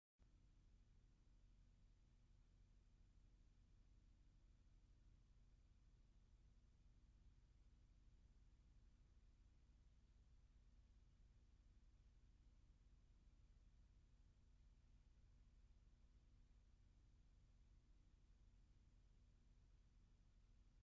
Hornets From Hell
Hornets attack bees outside a beehive.